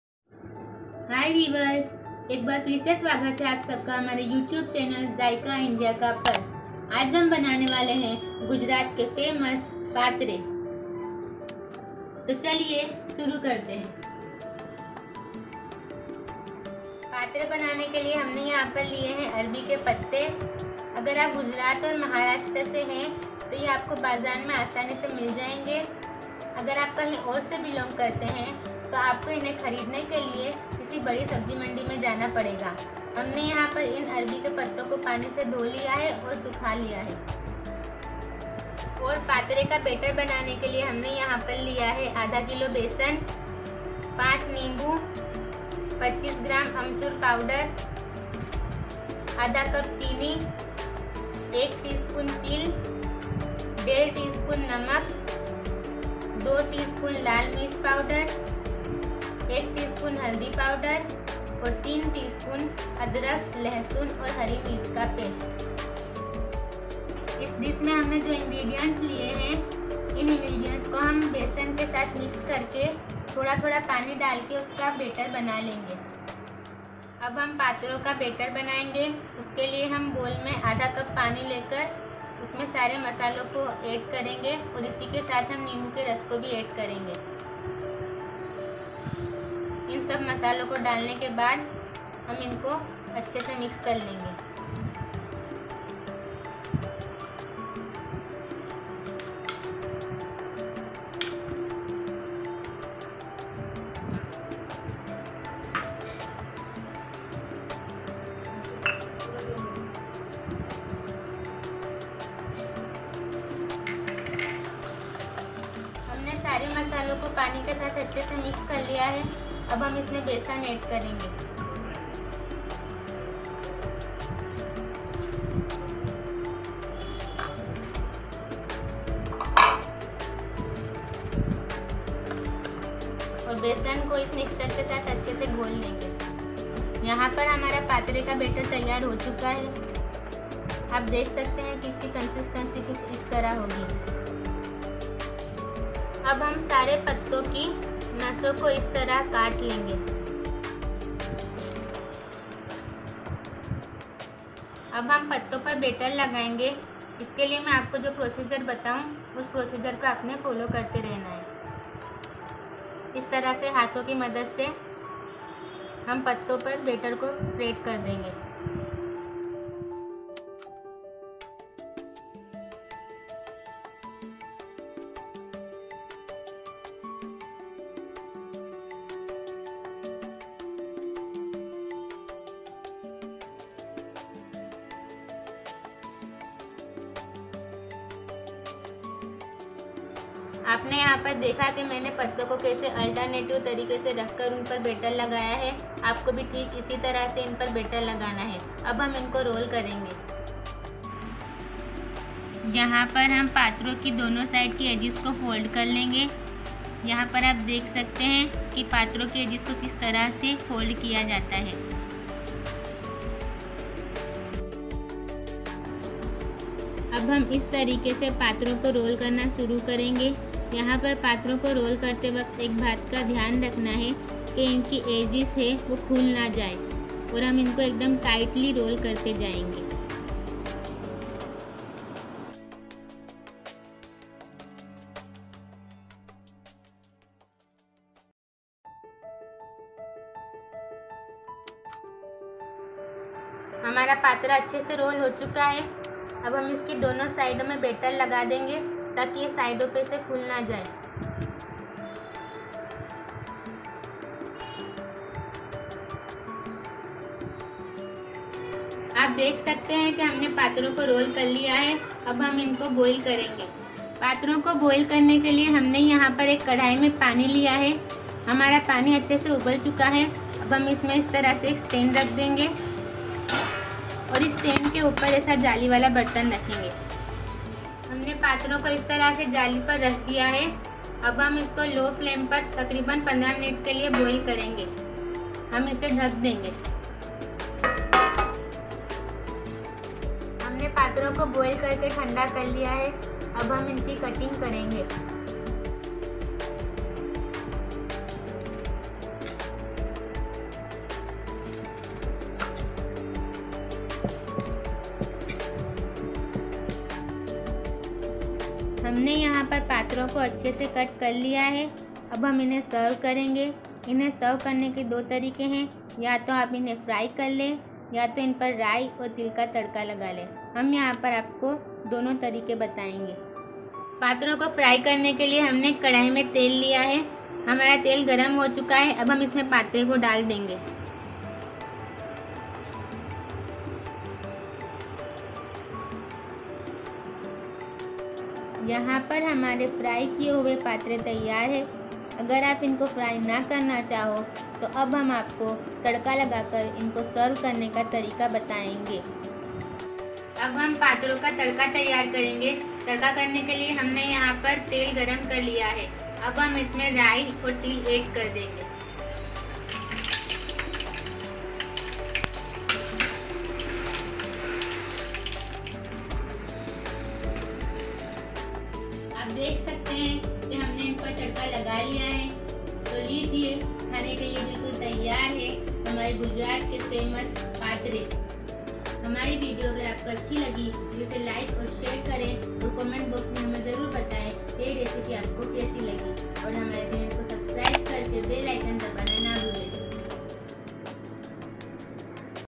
How to make Gujarati Patra | गुजराती पातरा कैसे बनाते हैं | Gujarati Patra recipe in Hindi
How to make patra,
how to make patra at home,
bardoli (gujarat) ke famous patra kaise banaye

गुजराती पातरा के लिए सामग्री

अरबी के पत्ते - 15-20
बेसन - 500 ग्राम
नींबू - 5
आमचूर पाउडर -25 ग्राम
चीनी - आधा कप
तिल - 1 छोटा चम्मच
हल्दी पाउडर - 1 छोटा चम्मच
अदरक-लहसुन हरी मिर्च का पेस्ट - 3 छोटी चम्मच

Ingredients for Gujarati Patra

Colocasia leaves - 15-20
Besan - 500 Gm
Lemon - 5
Aamchur Powder -25 Gm
Sugar - Half cup
Sesame seeds - 1 Tea spoon
Salt - 1.5 Tea spoon
Red chilli powder - 2 Tea spoon
Turmeric powder - 1 Tea spoon
Ginger garlic grinchily paste - 3 Tea spoon